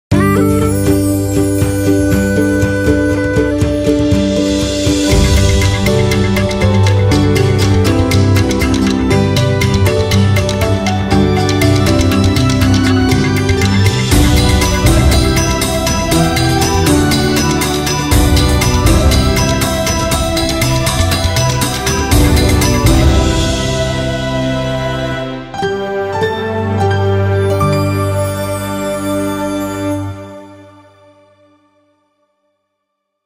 STATION ID INDOSIAR 2017, shot on Sony A7s
Station ID of Indosiar TV that I directed. Filmed using Sony A7s and DJI Inspire 1.
Location:
- Pelabuhan Sunda Kelapa, Jakarta
- Pantai Timang, Jogjakarta
- Lombok